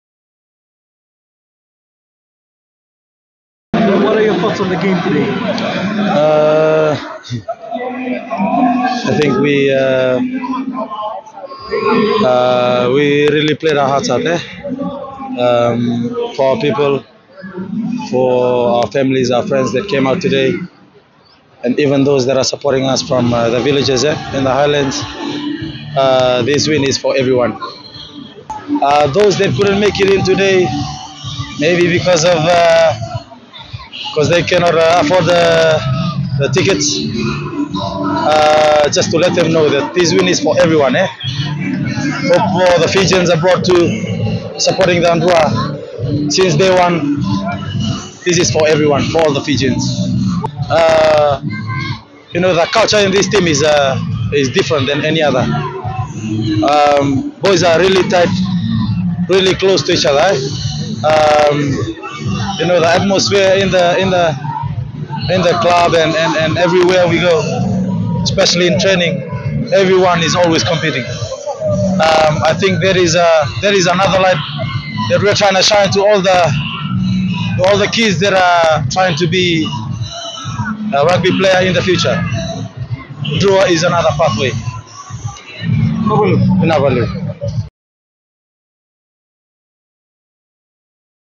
We played our hearts out – Lomani | 11/03/2023
Swire Shipping Fijian Drua halfback Frank Lomani says they really played their hearts out for all the Drua fans and families.

While speaking after the historic win, Lomani says this win is for everyone.